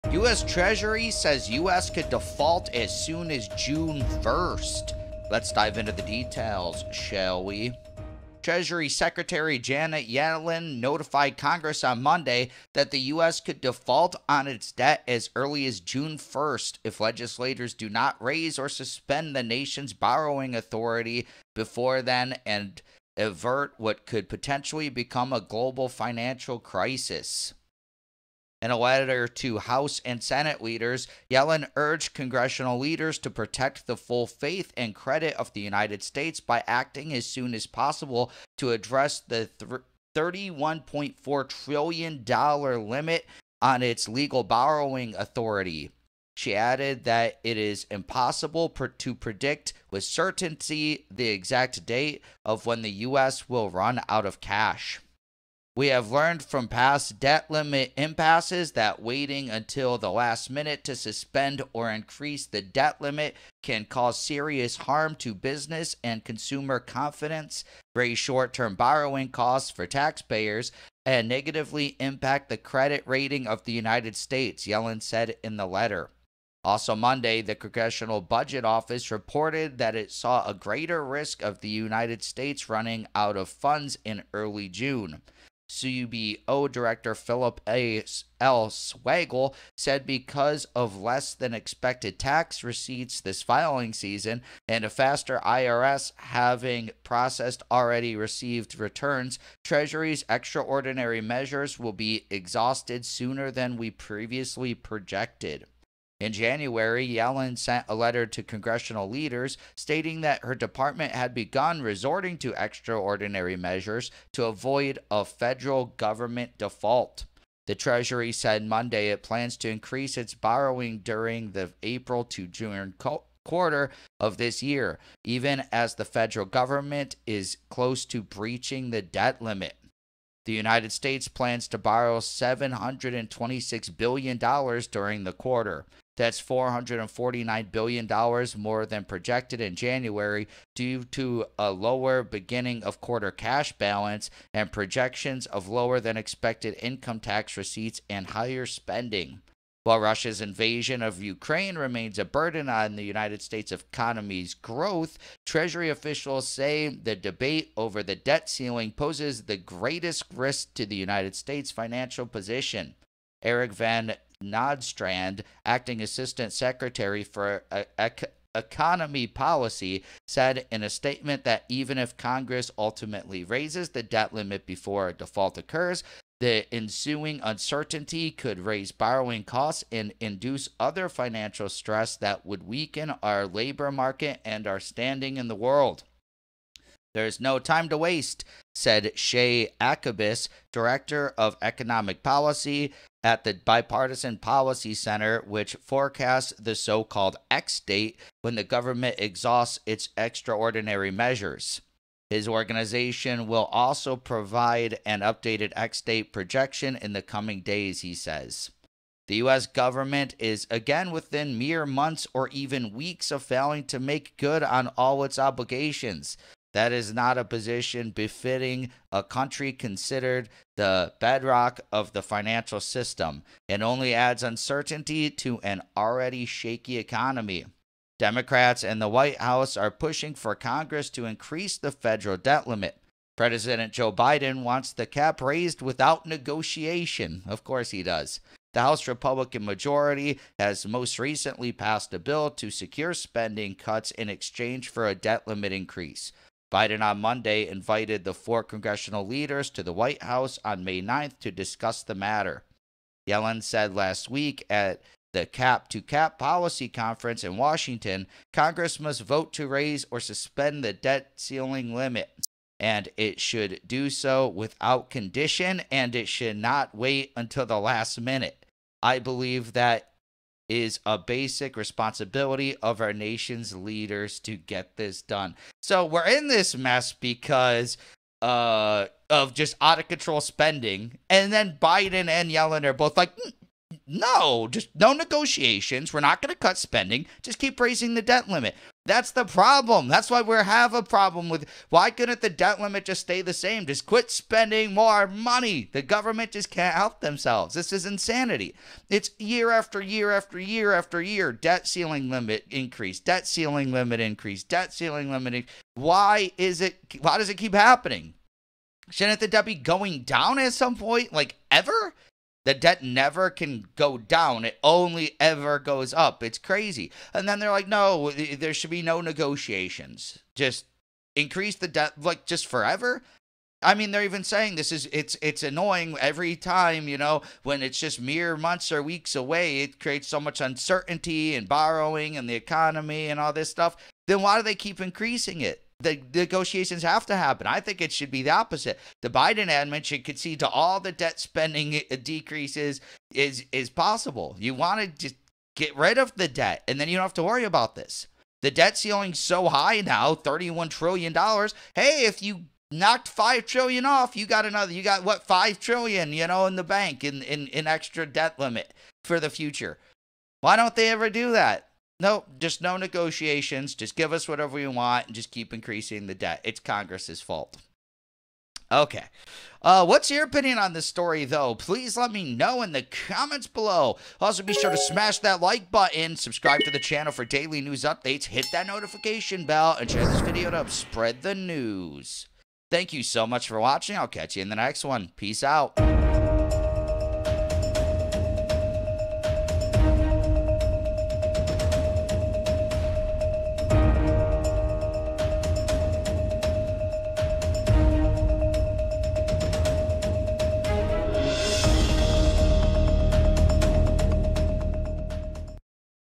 U.S. Treasury Says U.S. Could Default On Debt As Soon As June 1
Treasury Secretary Janet Yellen notified Congress on Monday that the U.S. could default on its debt as early as June 1, if legislators do not raise or suspend the nation’s borrowing authority before then and avert what could potentially become a global financial crisis.